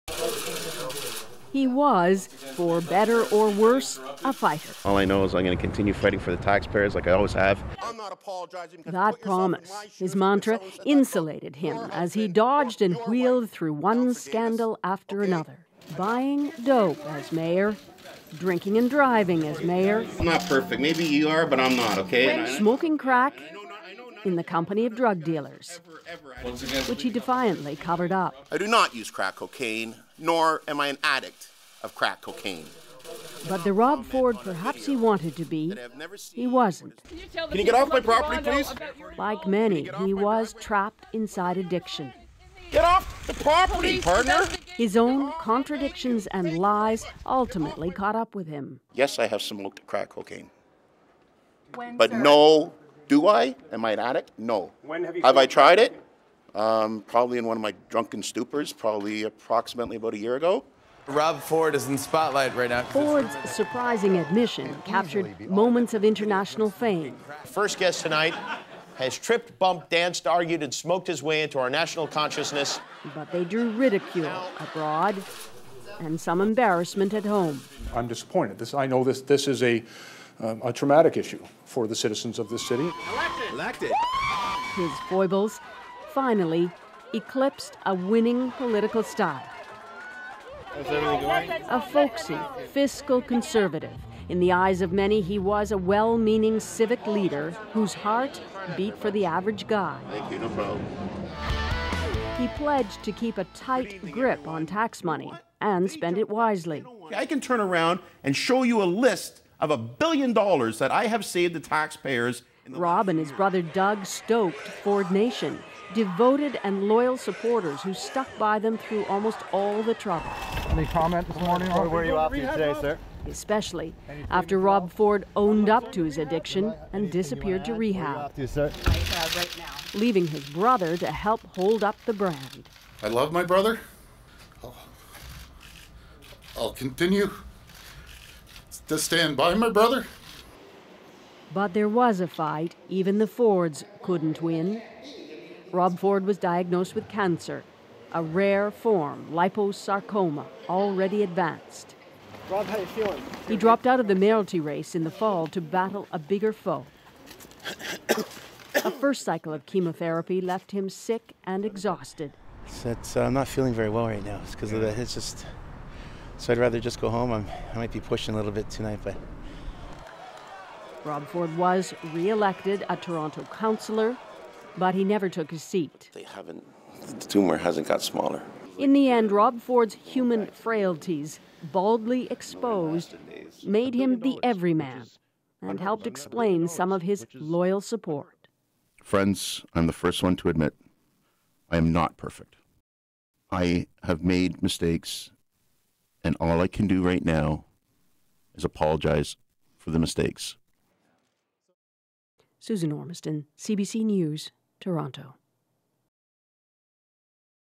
Rob Ford Dies
Former Toronto mayor Rob Ford has died of cancer at the age of 46.